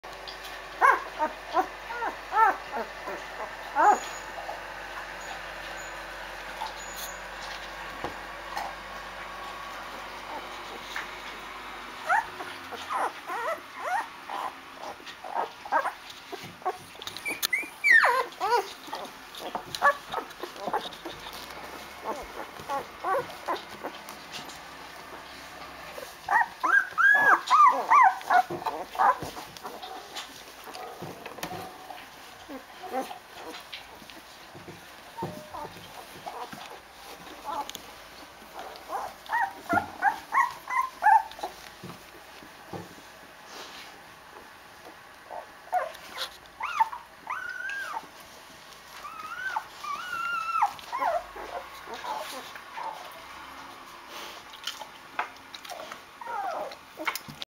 Rhodesian Ridgeback Puppies Day 8 | E-Litter
Sleepy puppies are good puppies! lol Having our siesta with a full belly... but also starting to crawl around a lot in the litter box - today still in order to look for cuddling, soon we will do it looking for adventures!